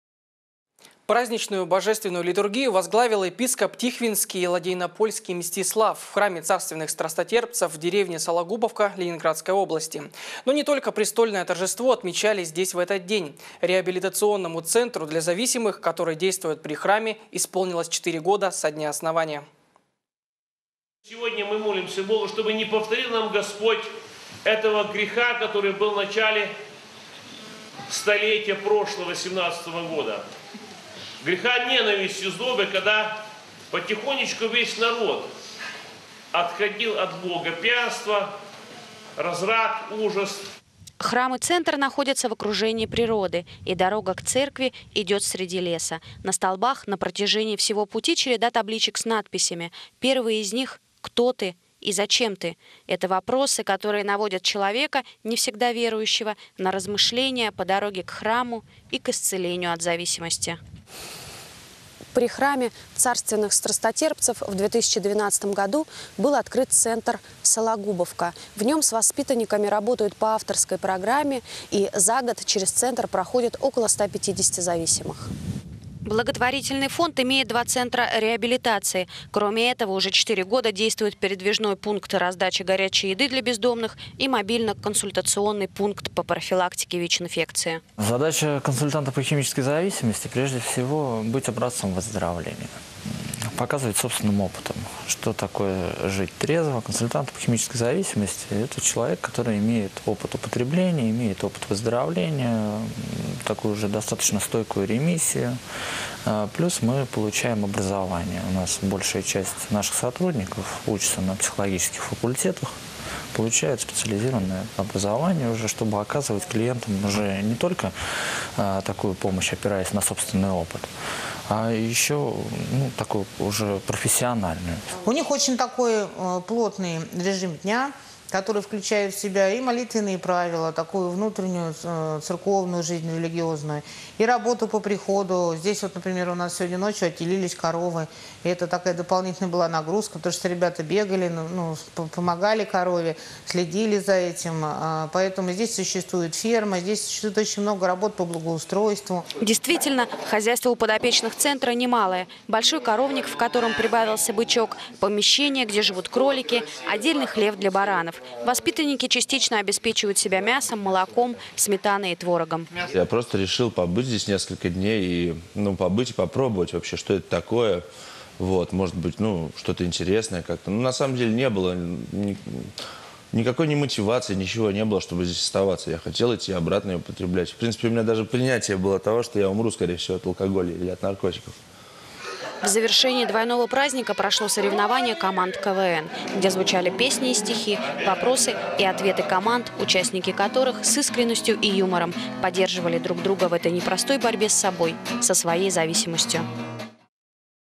4 года Центру для зависимых в д. Сологубовка
Праздничную Божественную литургию возглавил епископ Тихвинский и Лодейнопольский Мстислав в храме святых Царственных страстотерпцев в деревне Сологубовка Ленинградской области. Но не только престольное торжество отмечали здесь в этот день. Реабилитационному центру для зависимых, который действует при храме, исполнилось 4 года со дня основания.